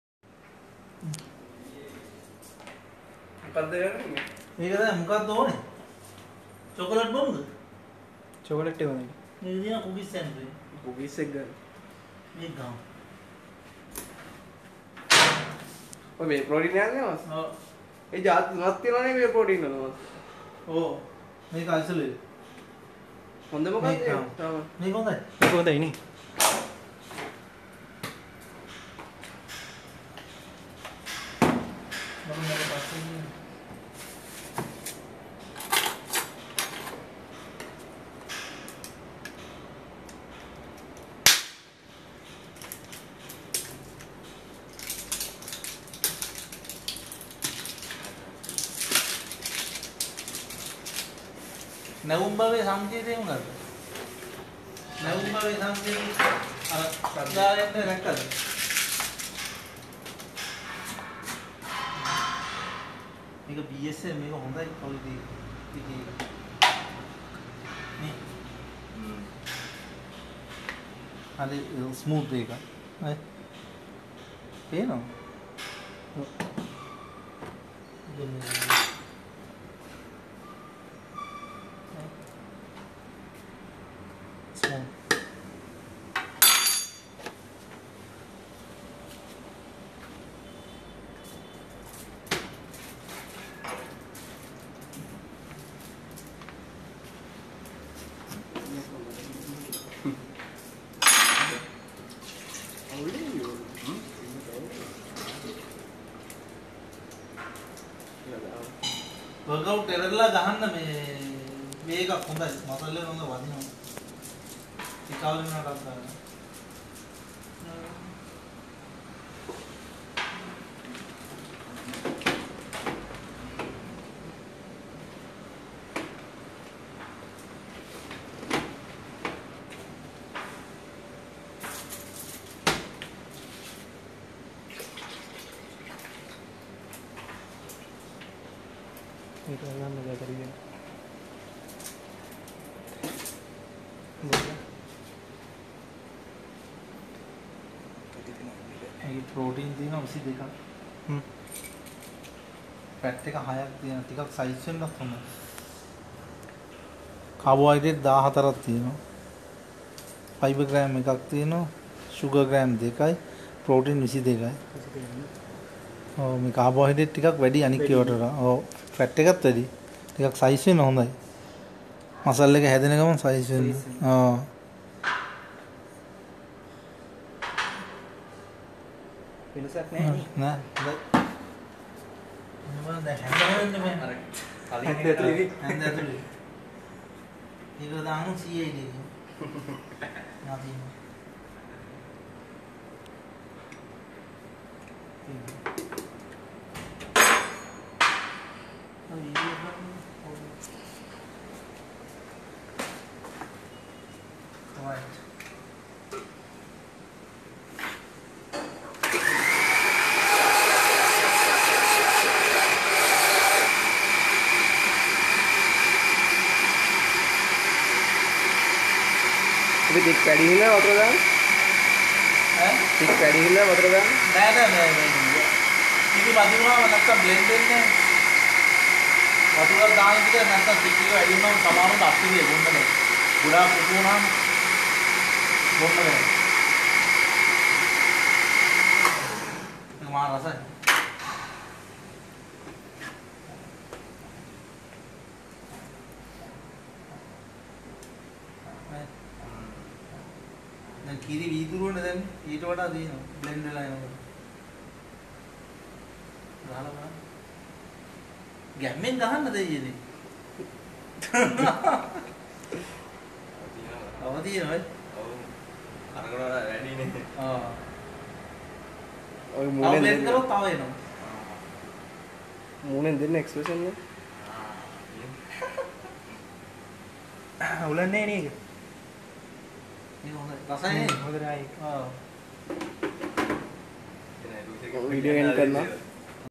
Choose a best protein supplement
# BSN Whey Protein කියන්නෙ  හොදට වැඩකරන whey protein එකක් .
# BSN Syntha 6 whey protein
# 2.27kg (48 servings )
# 1scoop (47g) per container
# calories 200
# total fat 6g
# total carbohydrates 14g
# fiber 1g
# total suger 2g
# protein 22g